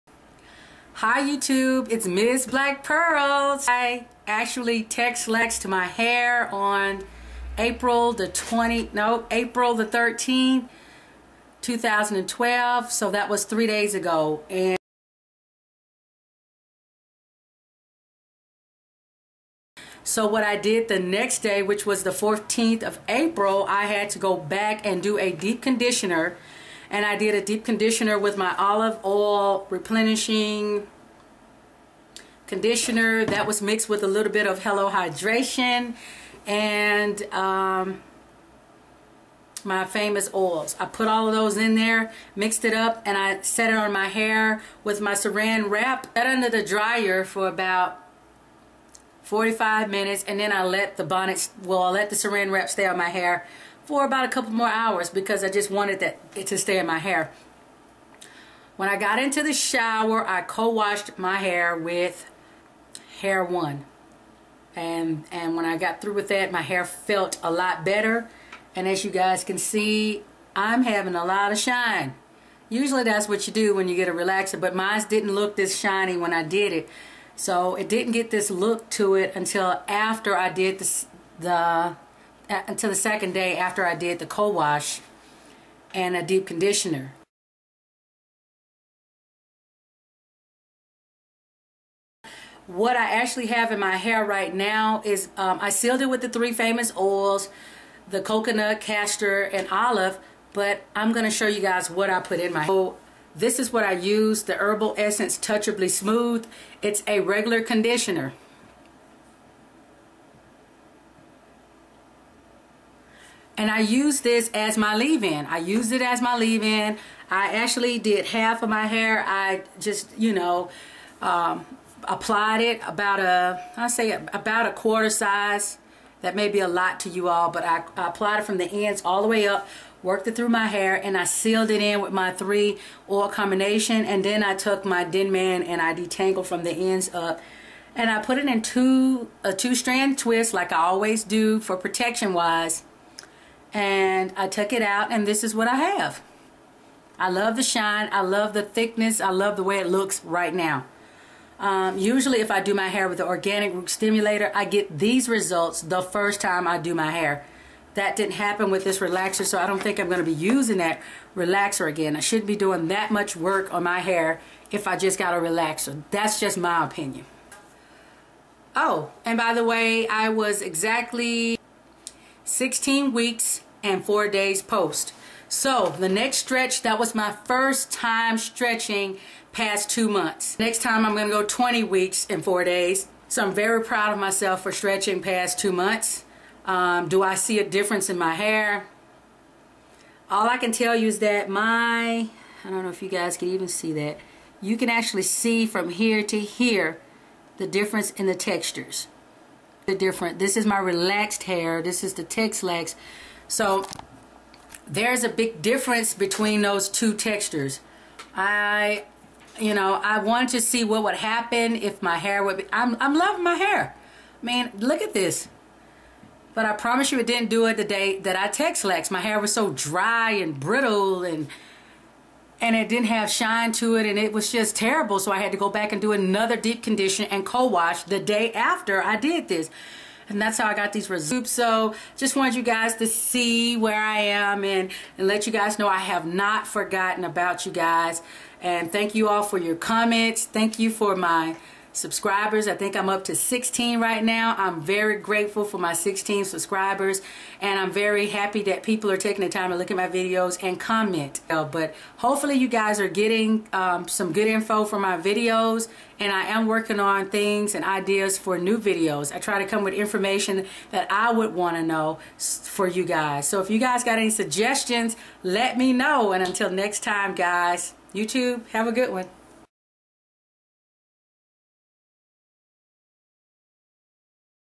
My come back from a cheap relaxer#26
Made the wrong choice,cheap aint always better I normally use ORS olive for normal hair but they have gone up on the price $9.47 What happen to $6.12. Im on a budget guys,
First time texlaxing here, so those shiny ends you see are relaxed bone straight I texlaxed my newgrowth. This was my 1st 16 wk stretch